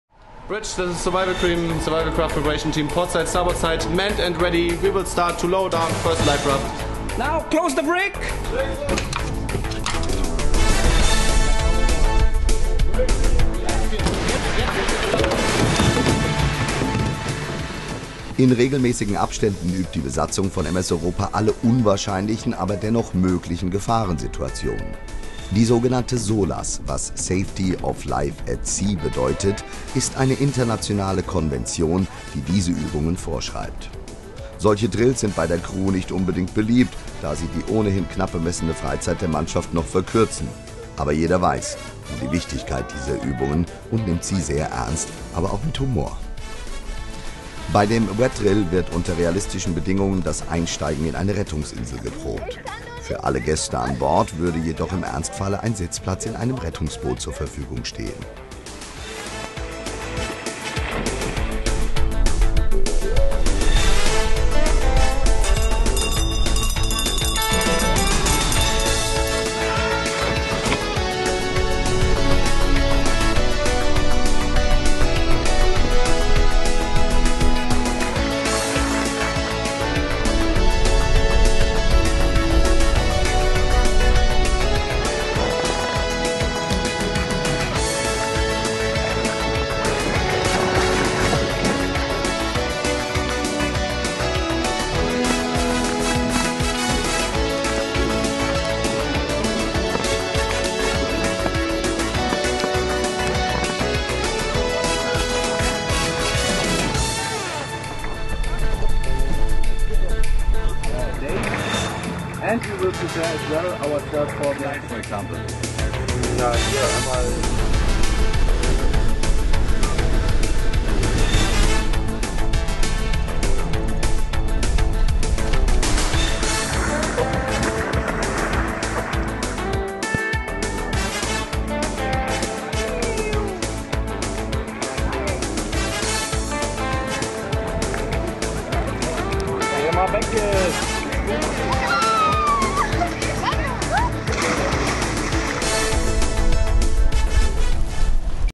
Basic Safety Training on cruiseship
Das Basic Safety Training ist eine Vorraussetzung für die Arbeit auf Kreuzfahrtschiffen.